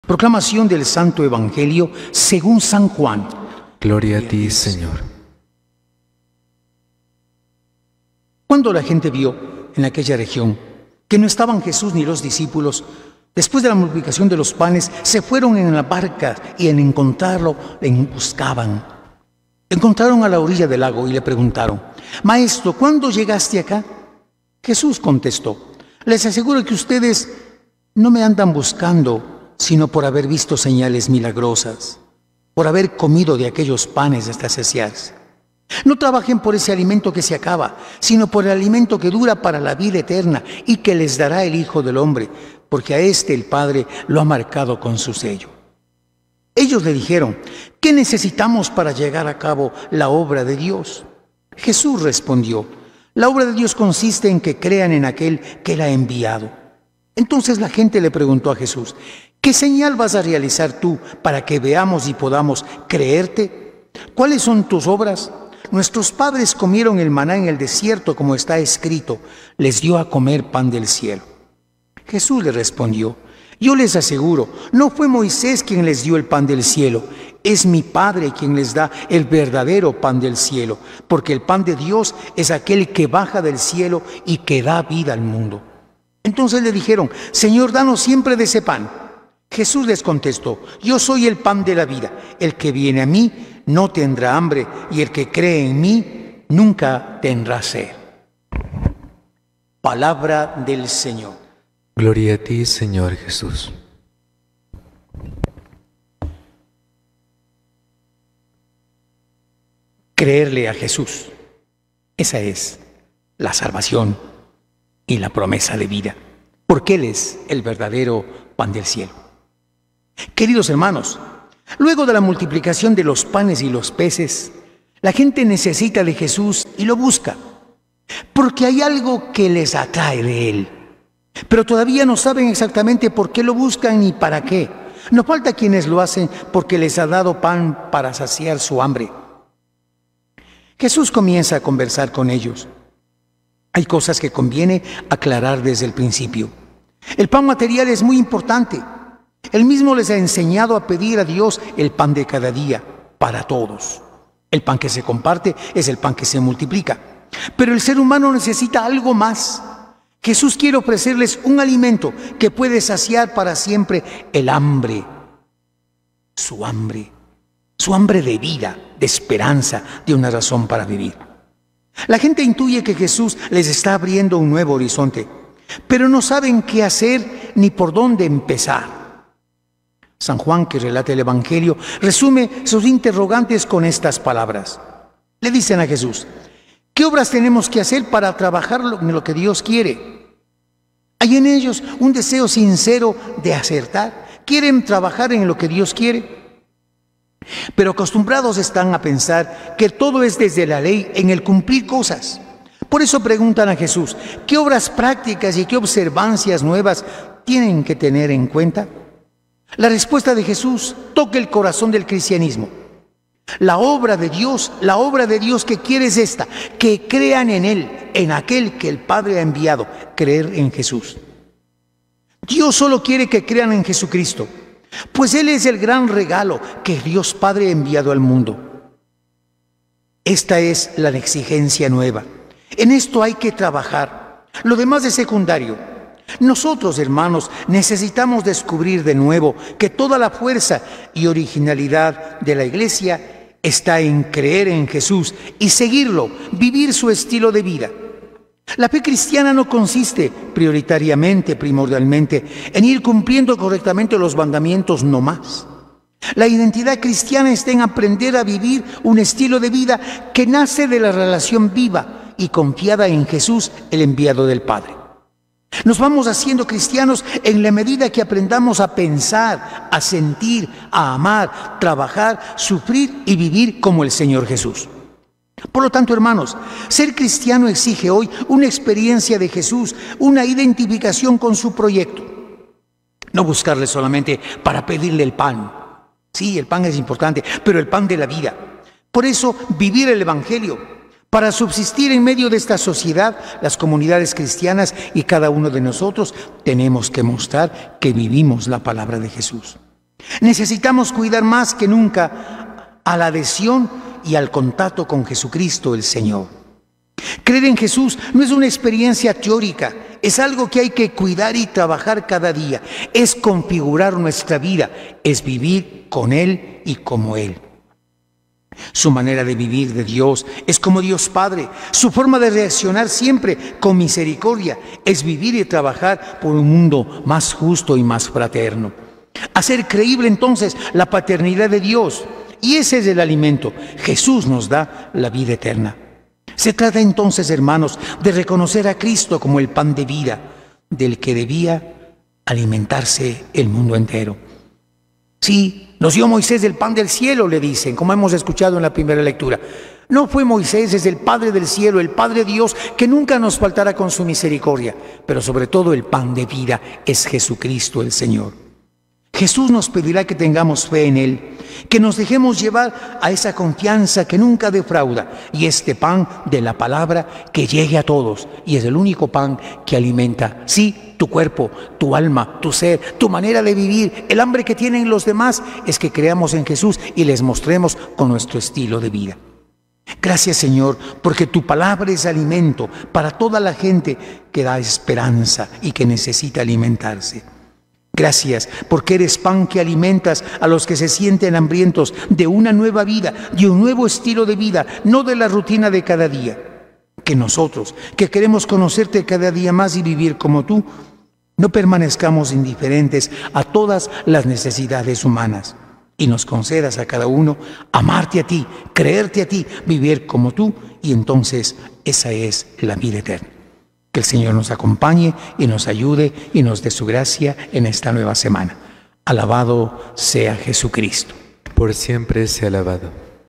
Evangelio y homilía de hoy | Domingo 4 de agosto de 2024
INFORMACIÓN
▶ Les ofrecemos el evangelio y homilía de hoy.
▶ Desde la Catedral de Ibarra - Ecuador
▶ Preside Mons. René Coba Galarza, Obispo de Ibarra

CRONOLOGÍA
00:00 Evangelio
01:45 Homilía